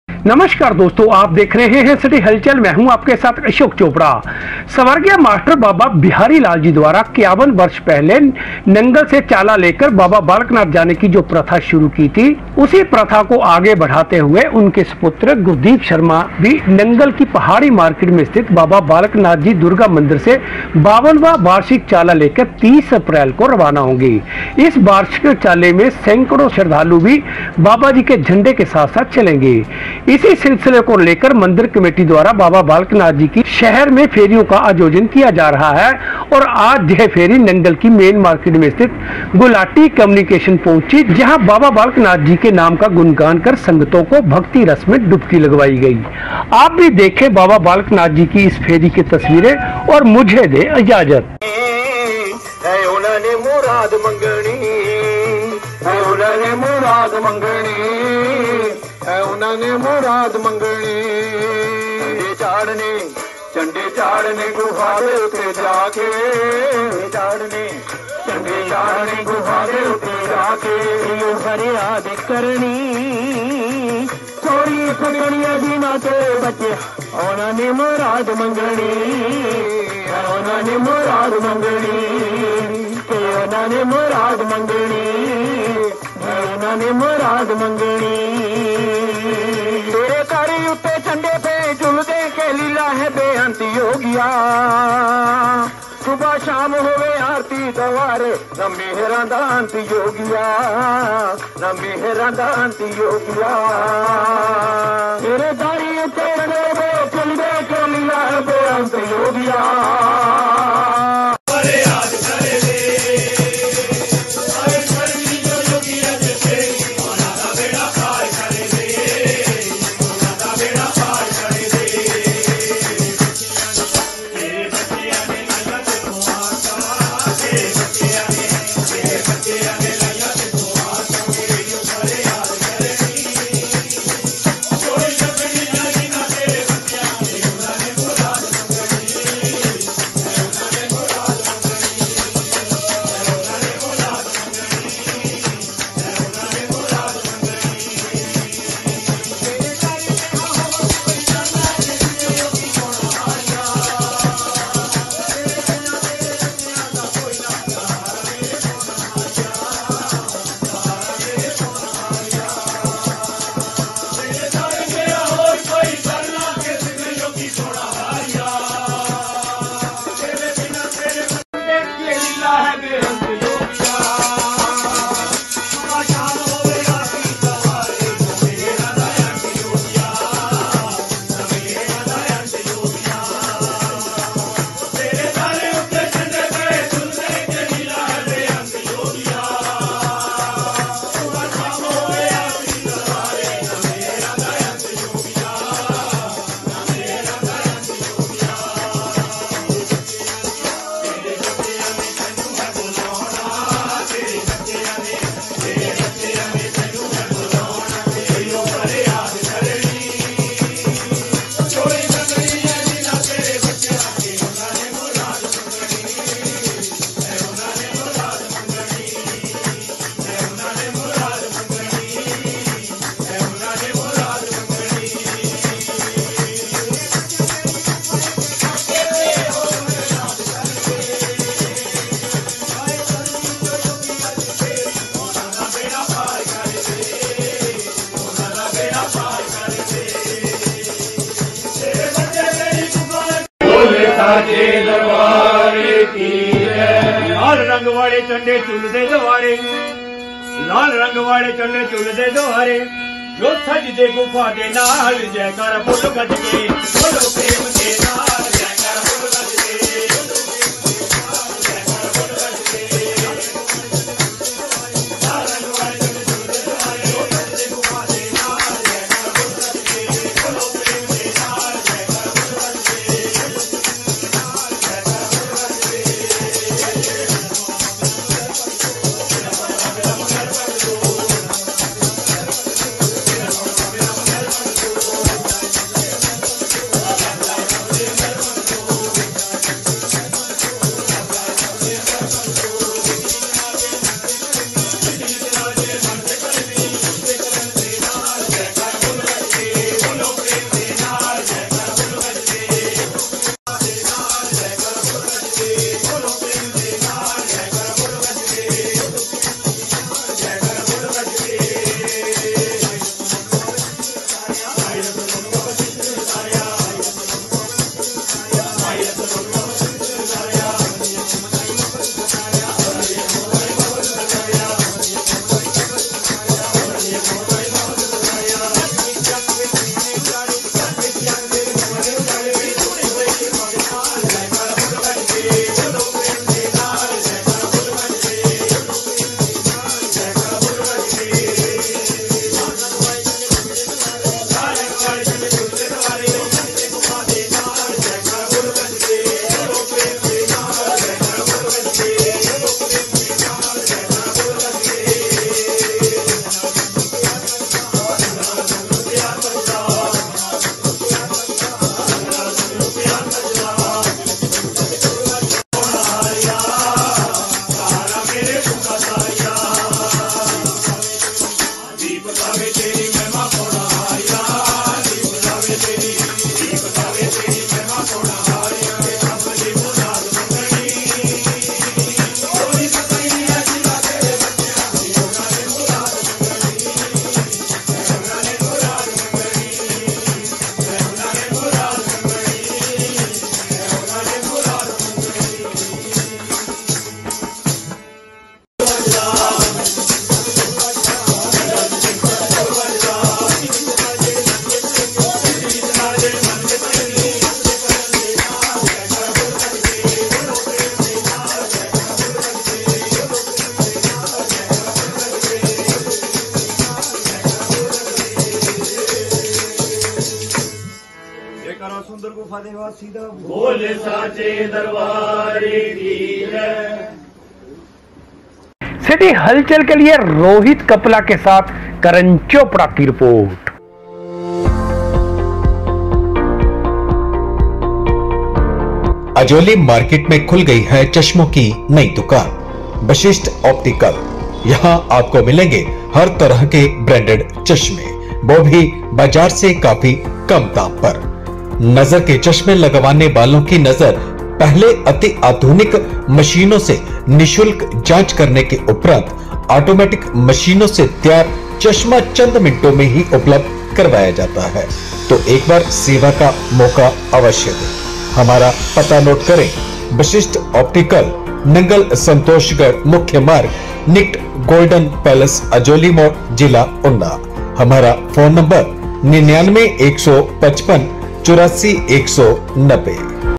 नंगल से बाबा बालक नाथ जी के दरबार के लिए रवाना होने वाले चाले के लिए निकाली जाने वाली फेरीओ से
नंगल से बाबा बालक नाथ जी के दरबार के लिए रवाना होने वाले चाले के लिए निकाली जाने वाली फेरीओ से महौल हुआ भक्ति में देखें पूरी रिपोर्ट